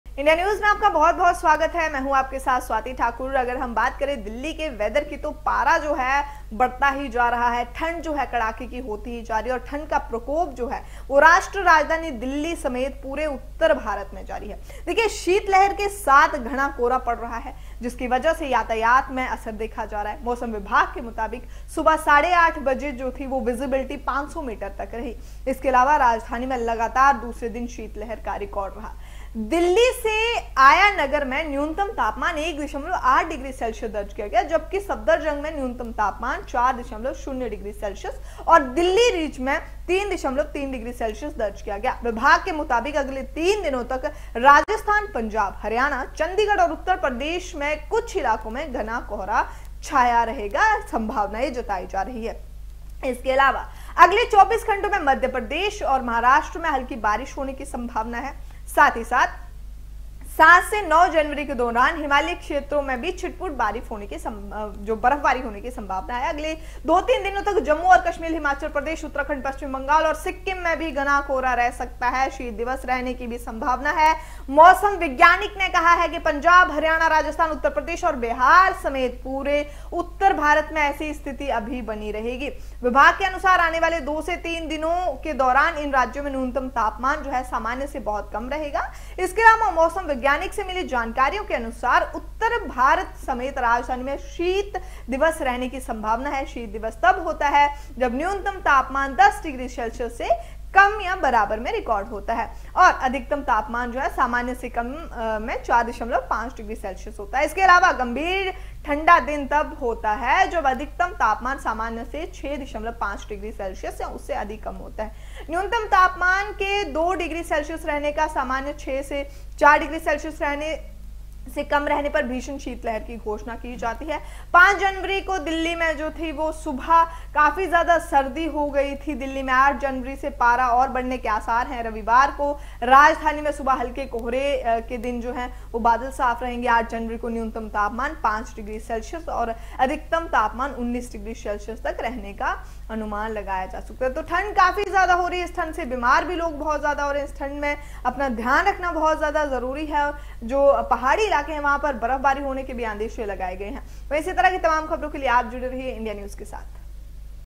दिल्ली में 1.8 डिग्री दर्ज हुआ पारा, यूपी से लेकर राजस्थान तक,जानें पूरे उत्तर भारत के मौसम का अपडेट
India Weather Update: दिल्ली में 1.8 डिग्री दर्ज हुआ पारा, यूपी से लेकर राजस्थान तक, जानें पूरे उत्तर भारत के मौसम का अपडेट